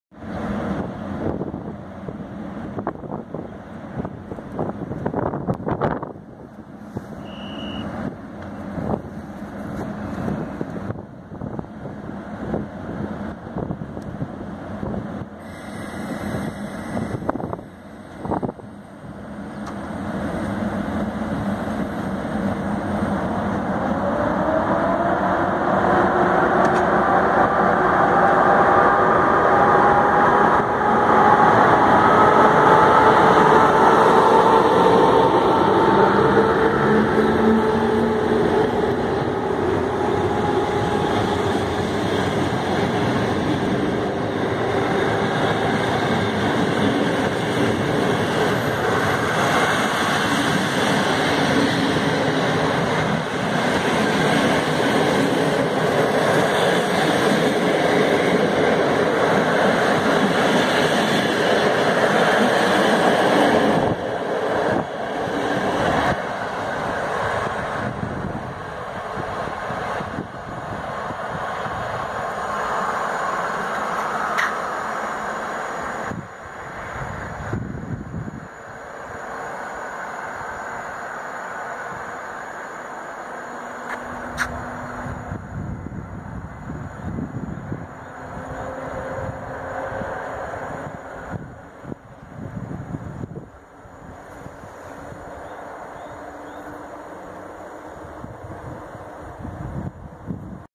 [24.04.16] Odjazd EP07-339 z opóźnionym składem TLK 26104/5 Sztygar
24 Kwiecień 2016 Pionki Zachodnie
Odjazd EP07-339 z opóźnionym o 10 minut składem TLK 26104/5 Sztygar rel. Lublin (12:20) - Pionki Zachodnie (13:26 [+13] / 13:27 [+13]) - Wrocław Główny (19:22).